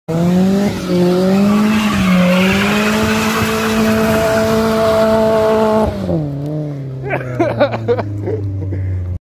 KIA Koup just ripping it up!! @ SA Torque Breakfast Run
an out take from my latest video of the SA torque The spring run.